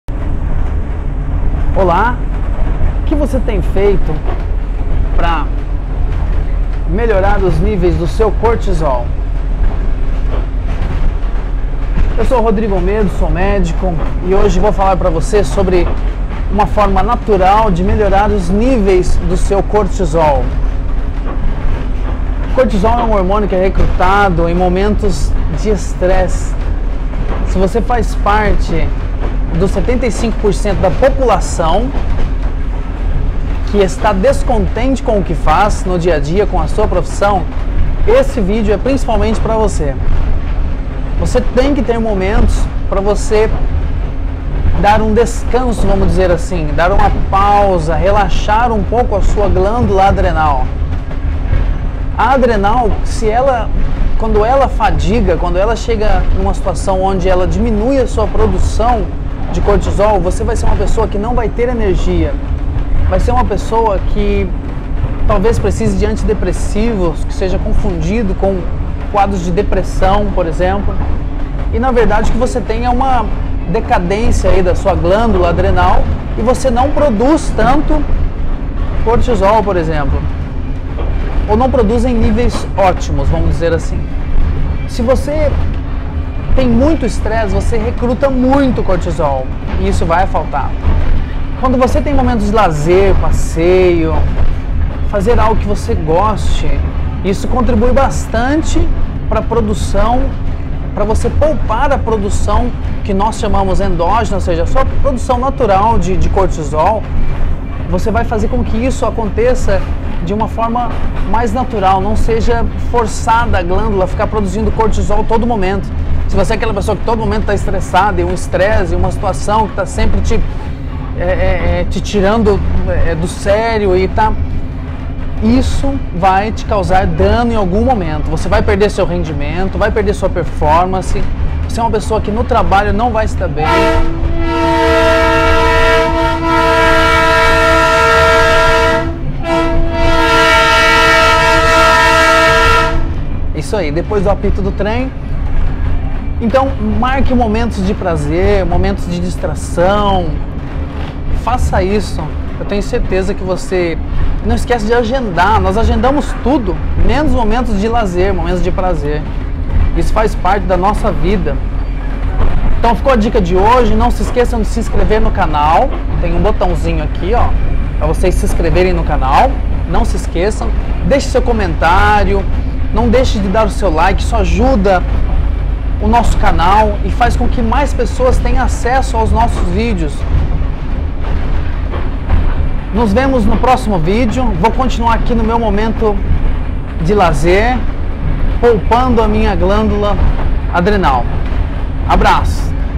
Falta de Energia, como o lazer pode te ajudar ?
Saiba como o estresse pode influenciar nos níveis do seu cortisol e entenda como o lazer pode contribuir para equilibrar esta balança